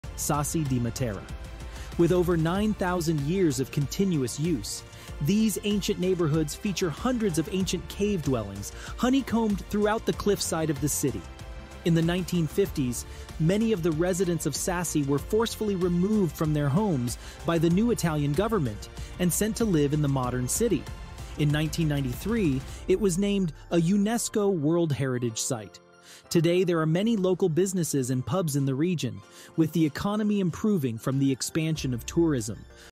These cave dwellings are over 9000 years old!
Sassi di Matera

With over 9000 years of continuous use, these ancient neighborhoods feature hundreds of ancient cave dwellings, honeycombed throughout the cliffside of the city. In the 1950s, many of the residents of Sassi were forcefully removed from their homes by the new Italian government and sent to live in the modern city. In 1993, it was named a UNESCO World Heritage Site. Today, there are many local businesses and pubs in the region, with the economy improving from the expansion of tourism.